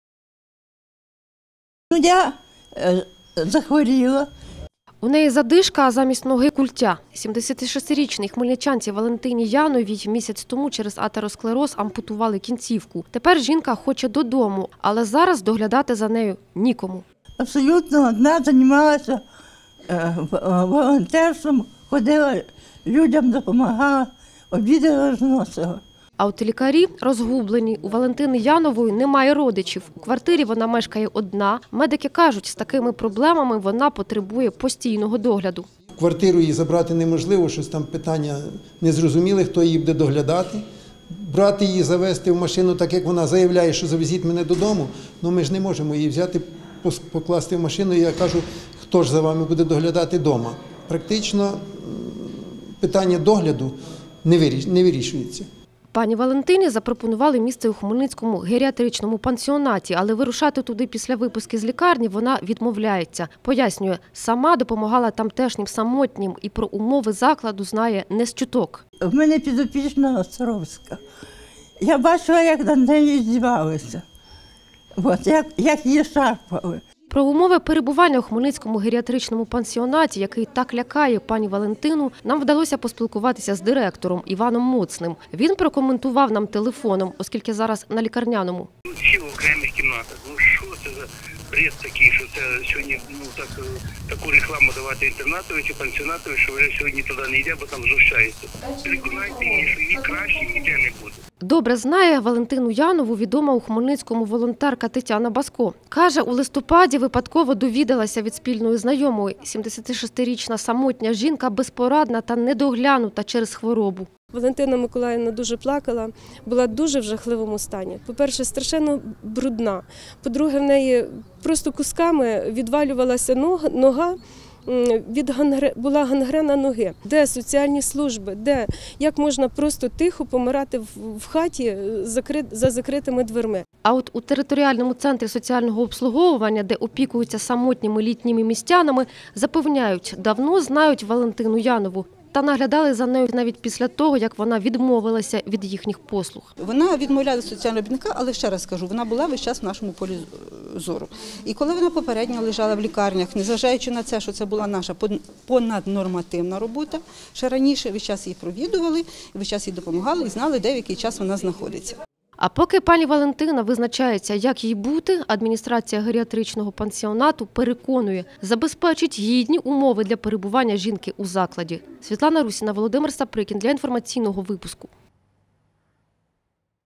Самотня хмельничанка з ампутованою ногою боїться перебування у геріатричному пансіонаті
У Хмельницькій міській лікарні не знають, як виписати із хірургічного відділення пацієнта. Йдеться  про 76-річну  хмельничанку  Валентину  Янову, котрій майже місяць як ампутували кінцівку. Жінка тепер, за словами лікарів, потребує постійного догляду. Але робити це нікому.  На перебування ж у геріатричному пансіонаті Валентина Янова не погоджується.